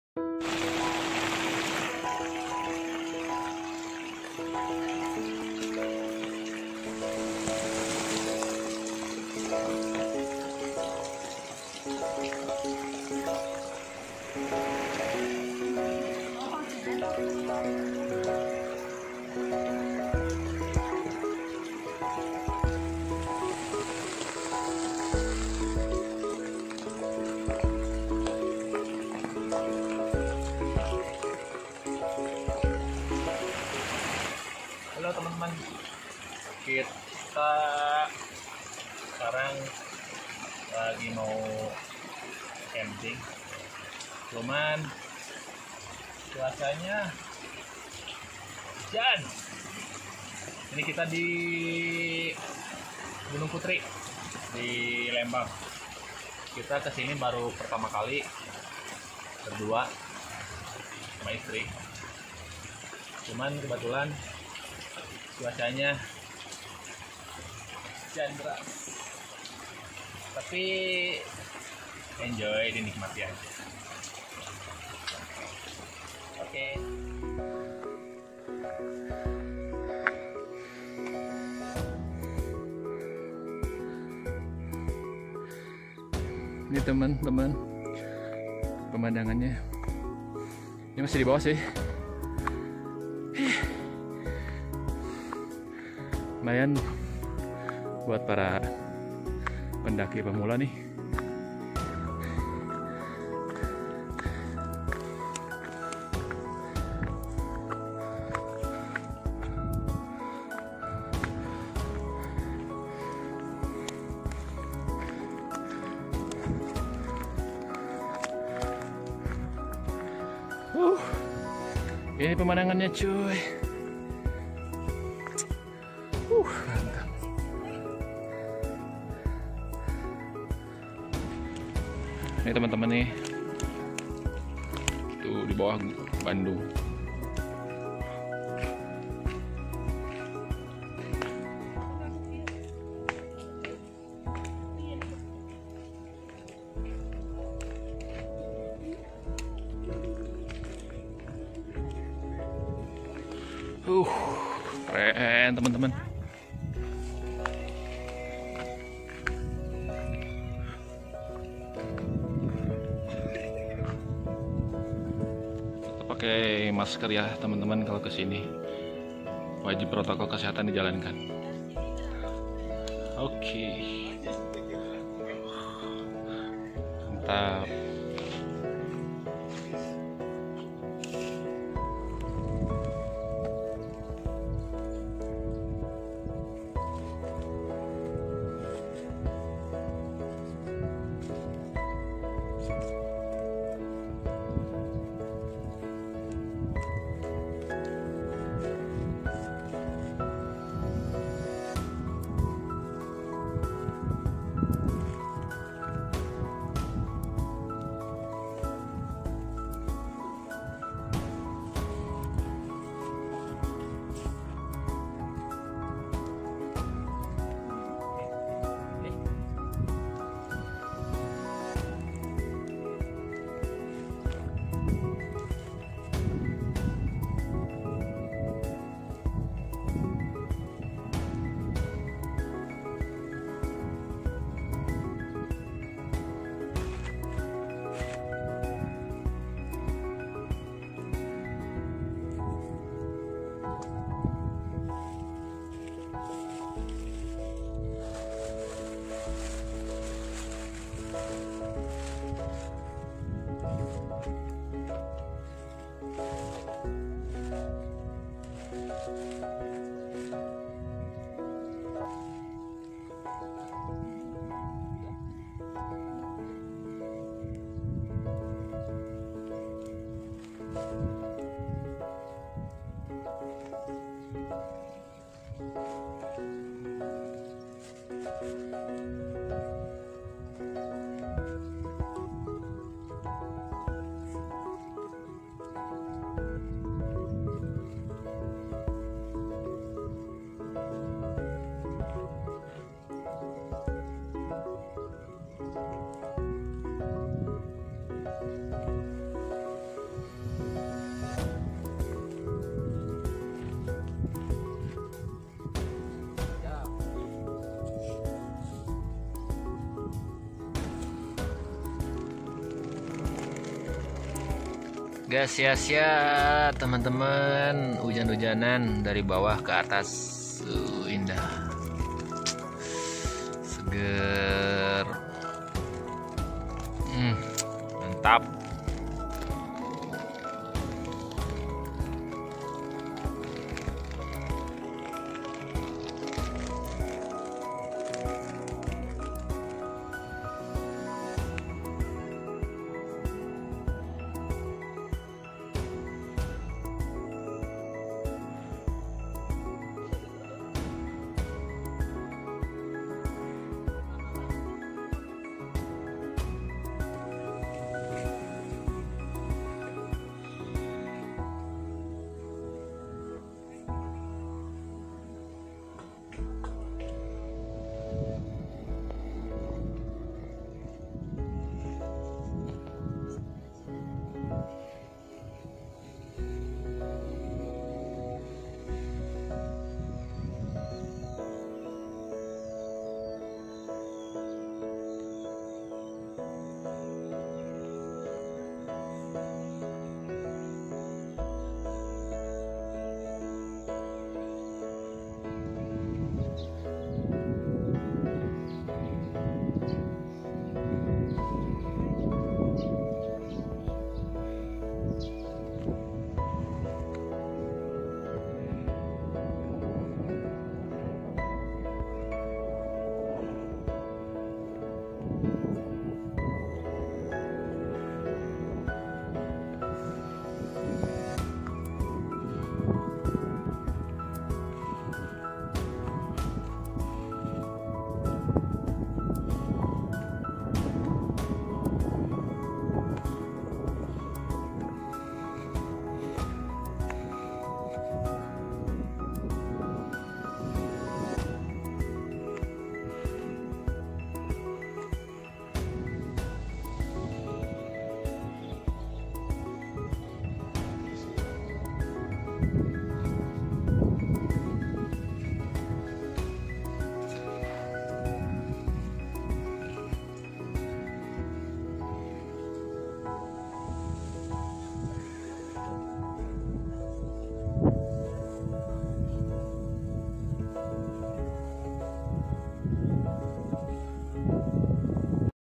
Camping Gunung Putri Lembang Bandung,Diguyur Hujan Deras Sekali !!
CAMPING GUNUNG PUTRI LEMBANG CUMA 17 RIBU SAJA MURAH BANGET

Record     : vivo v15 pro
Edit Video : video maker